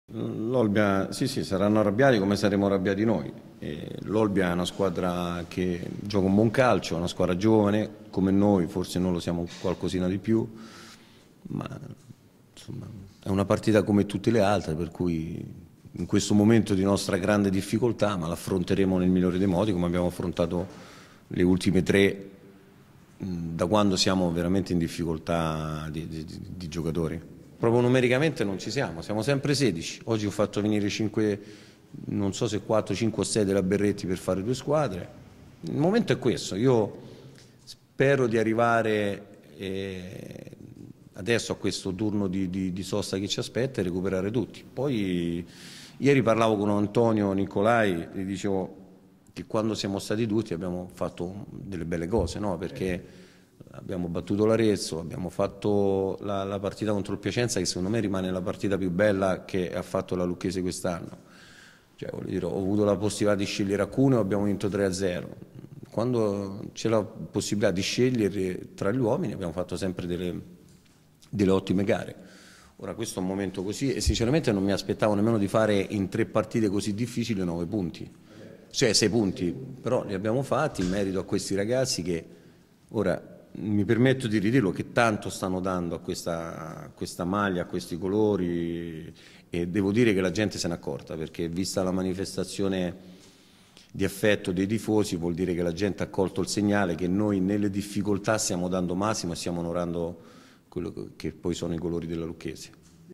Lopez presenta la trasferta di Olbia
CALCIO C - Giovedì pomeriggio conferenza stampa del tecnico della Lucchese in vista della difficile trasferta sarda. Rientra Fanucchi ma le asenze sono sempre numerose e pesanti.---

NoiTv